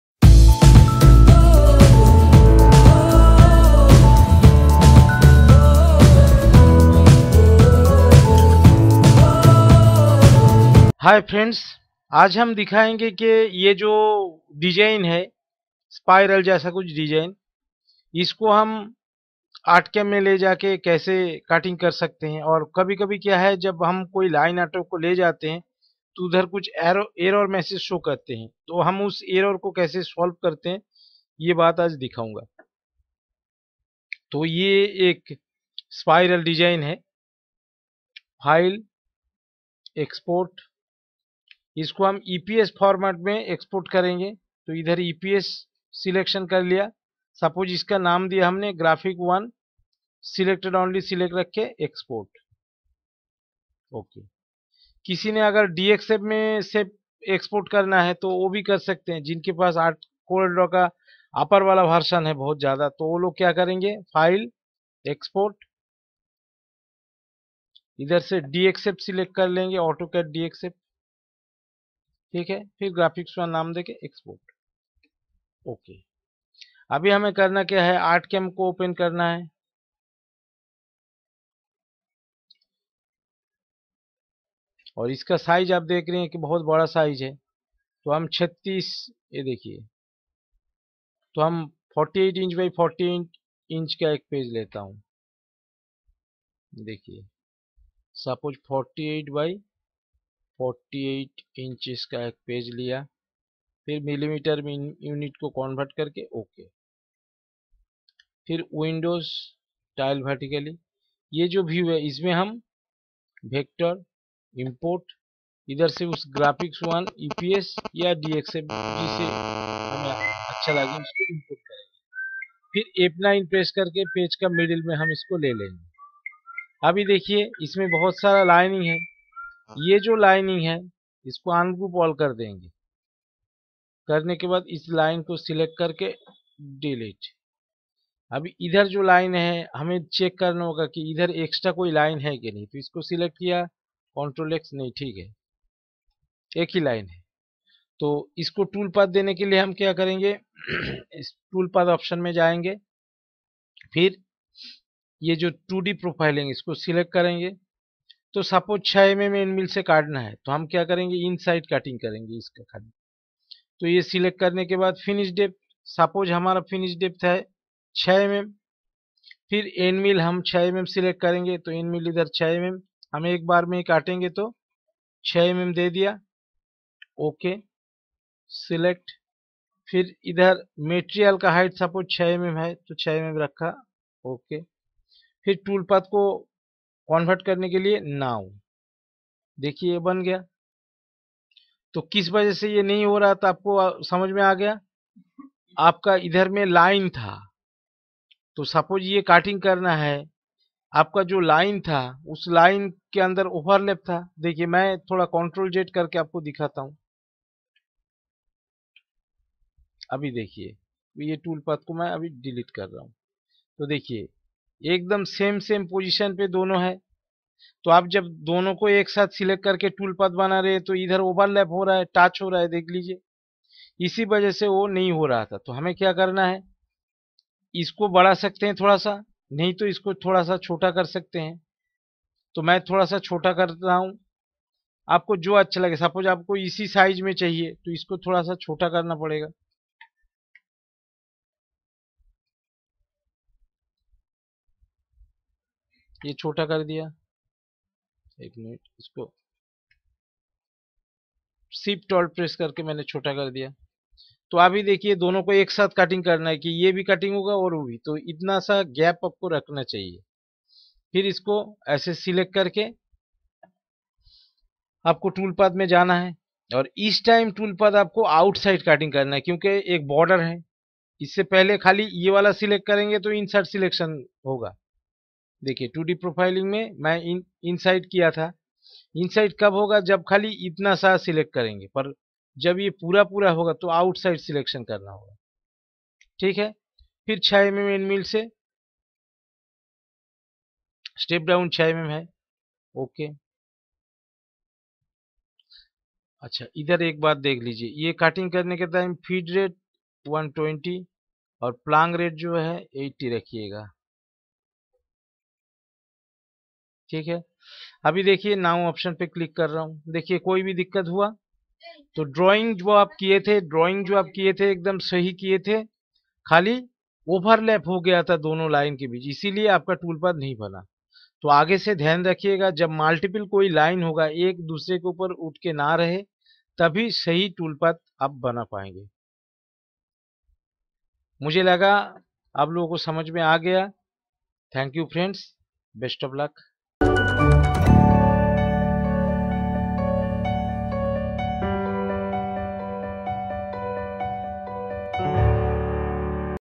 How to solve Errors of Vector drawing fault in ArtCAM 2008
In this video I have shown that how to solve the Errors of Vector drawing fault in ArtCAM 2008.